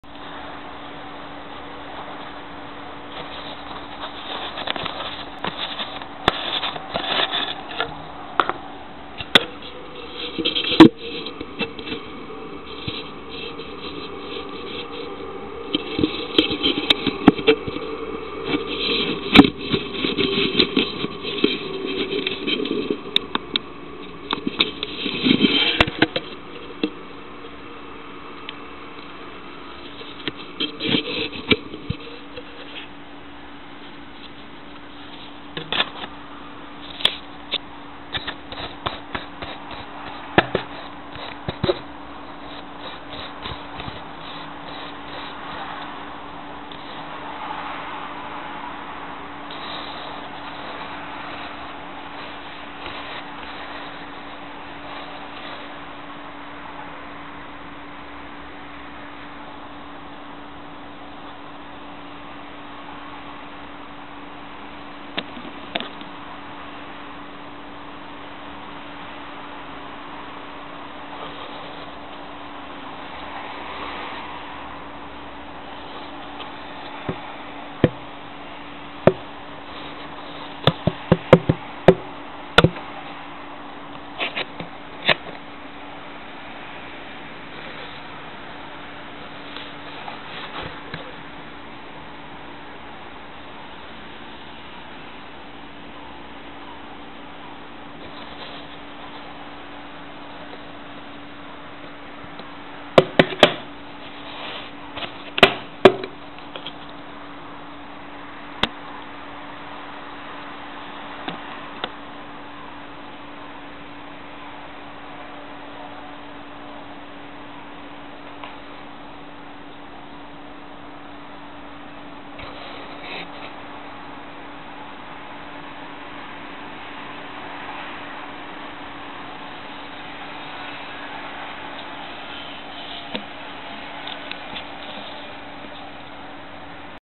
Night Butterflie
Night Butterflie - this moth hatches from the cocoon of the green caterpillar